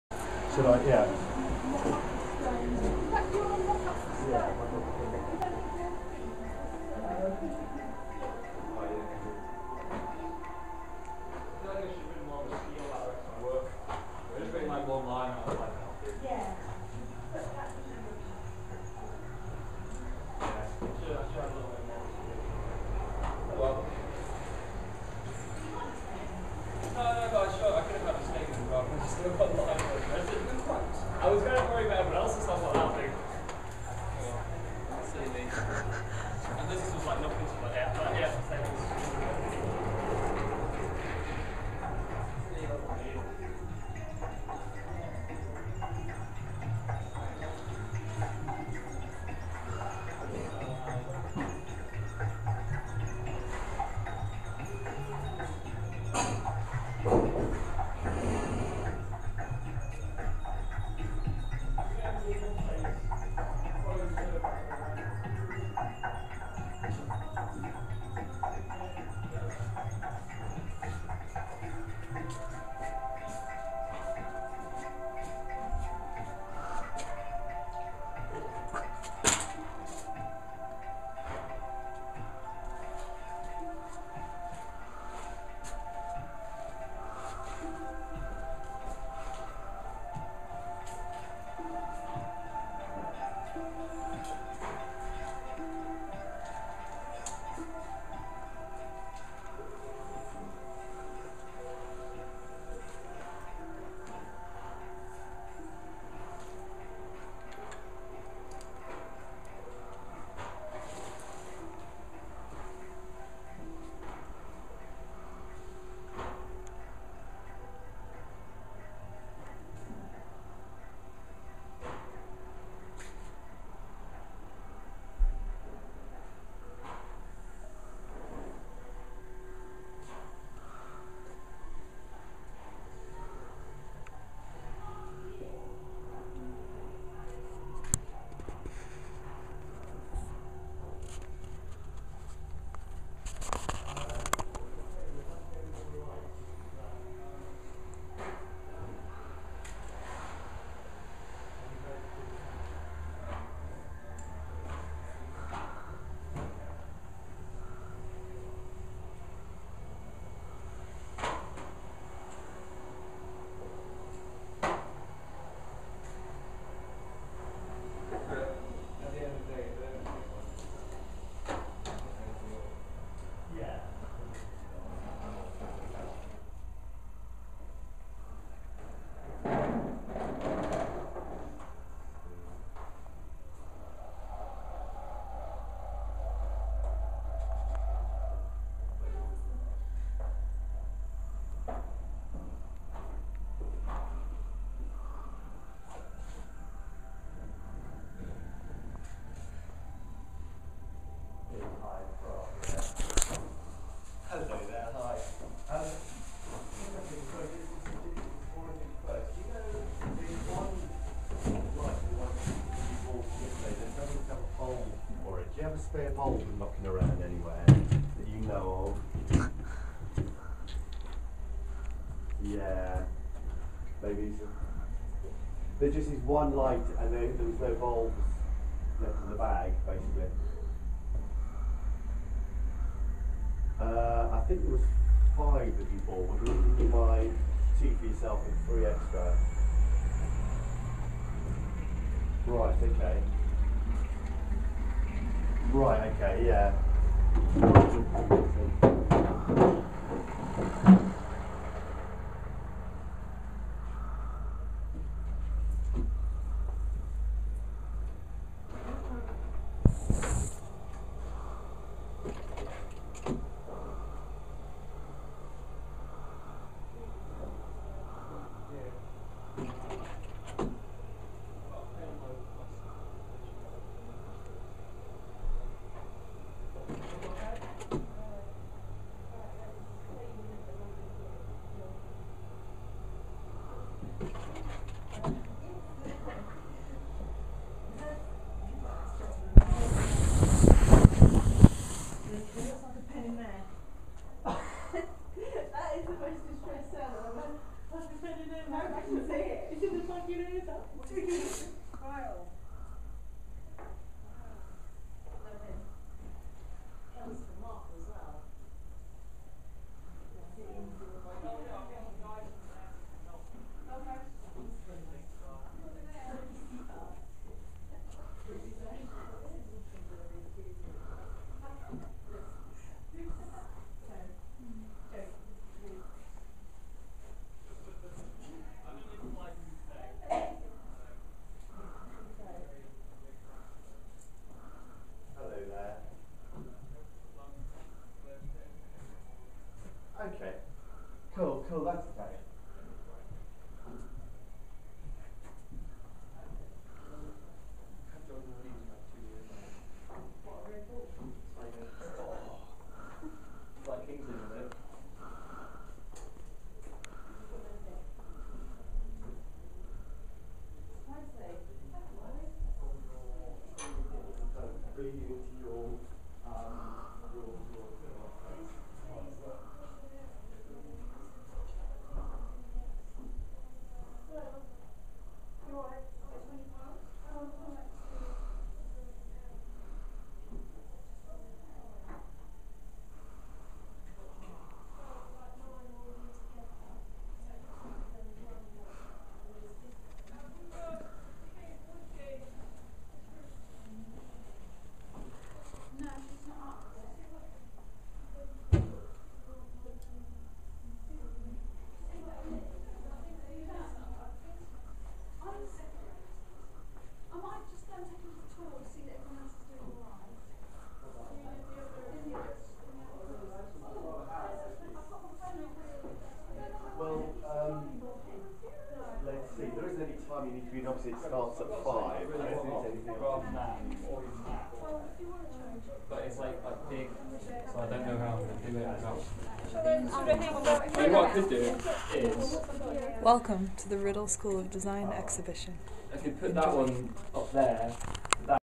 WSD Exhibition Walkthrough
About 2 hours before the Writtle School of Design 2nd and 3rd year exhibition went live, I made this video. Enjoy a taste of our crazy-cool smorgasbord of fine art and installations, and forgive the occasional badly-lit piece. Not all the lights were on!